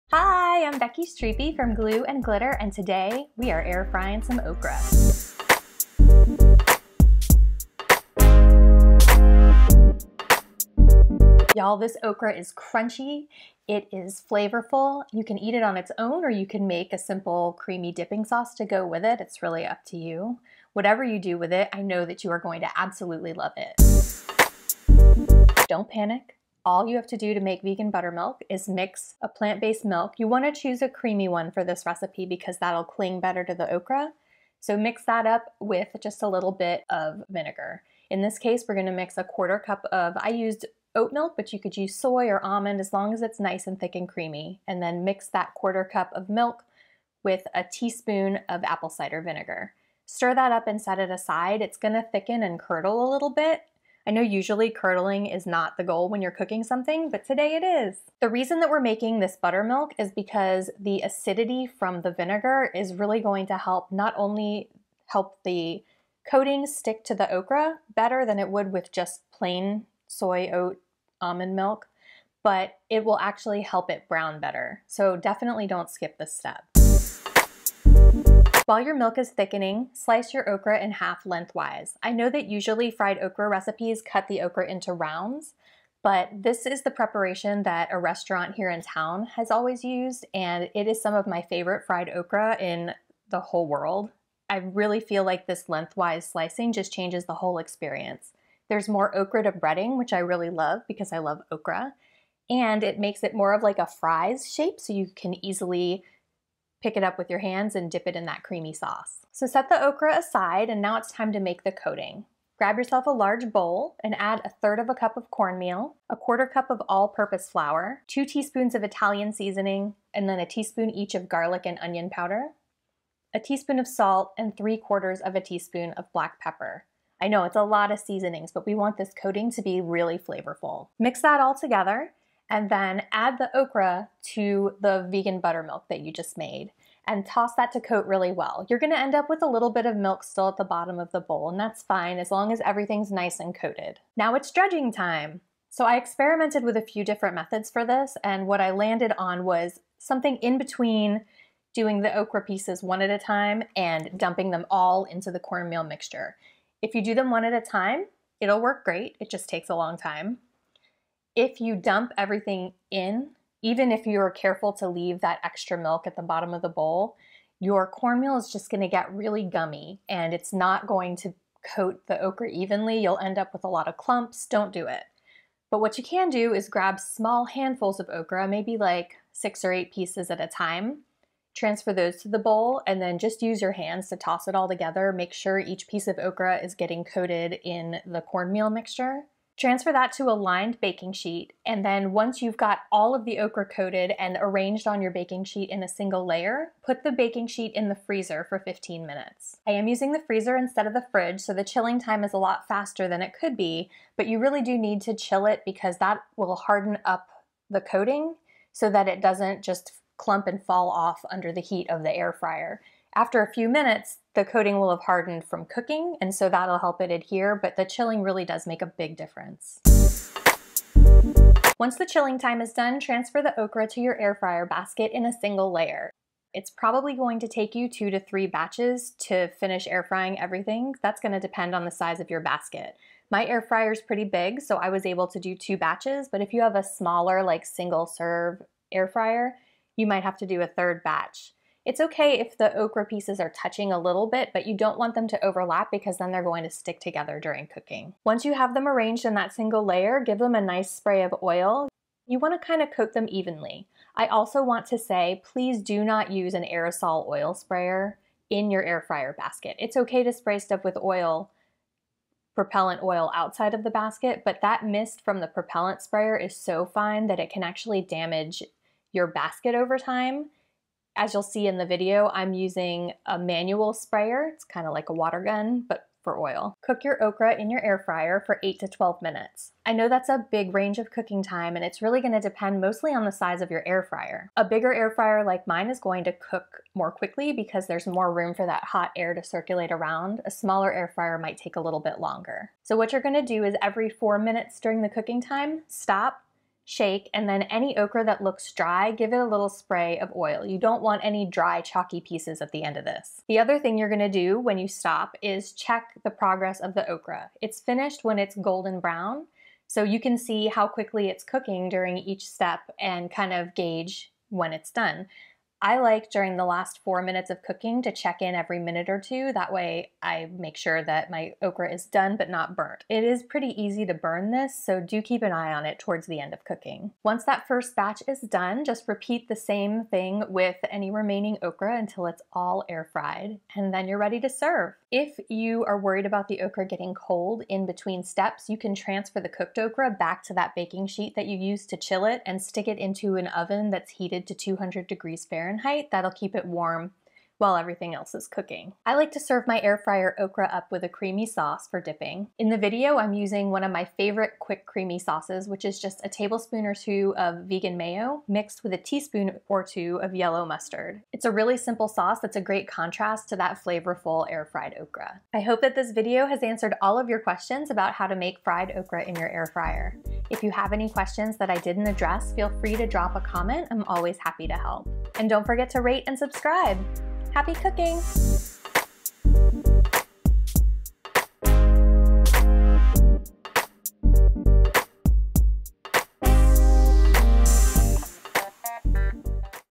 Crispy air fryer okra
This air fryer okra is crunchy, flavorful, and so easy to make! Just coat, freeze, and air fry to golden perfection. It was a recipe request from TG R., and I think you're going to love it!

💛Ingredients💛
1/4 cup soy milk or oat milk
1/4 pound fresh okra
1/3 cup cornmeal
1/4 cup all-purpose flour
2 teaspoons Italian seasoning
1 teaspoon garlic powder
1 teaspoon onion powder
1 teaspoon salt
3/4 teaspoon black pepper
spray oil

*Air Fryer
* Oil Sprayer